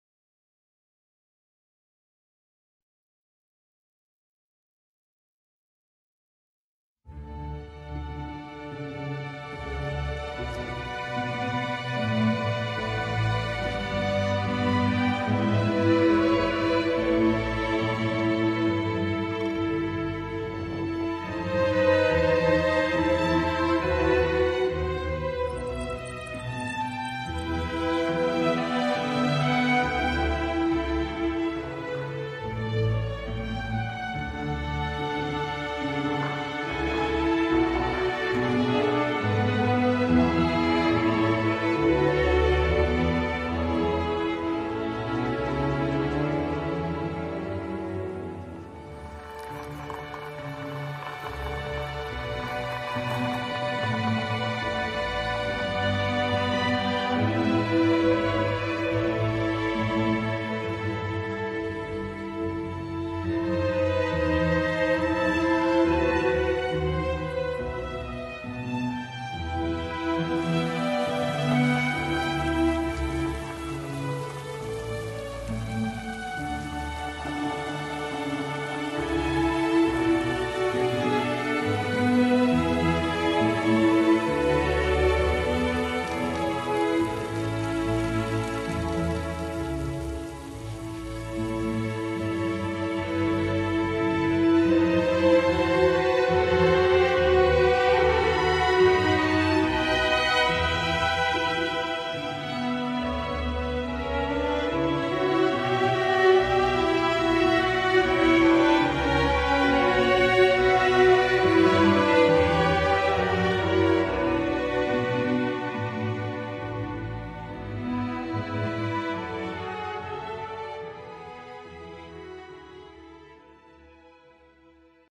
Bentley Continental Flying Star by Touring Superleggera
The 4th unit of the shooting brake built by Touring with the endorsement of Bentley Motors exhibits its pure elegance in the magnificent surroundings of a XVII century villa.